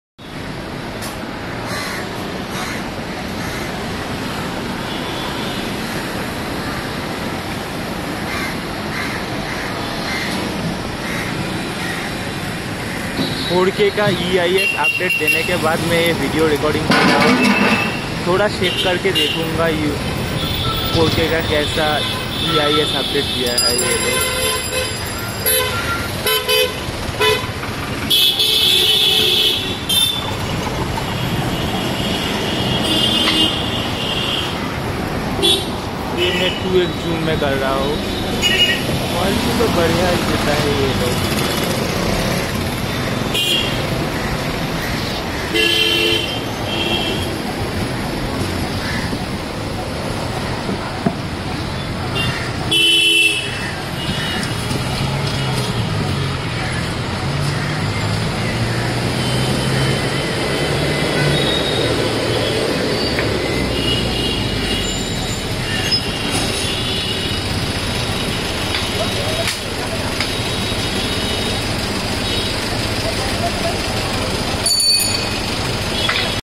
OnePlus 5 Camera 4K Video Shaking Test After EIS Update
After 4K EIS Update I Record This Videos. I Am Really So Much Shocked That I Shake My Mobile To Much But In This Record You Can't See Any Type Of Shaking..So Really OnePlus Working To Hard To Improve There EIS Stability..Now In OnePlus You Have Very Good Electronic Image Stabilization..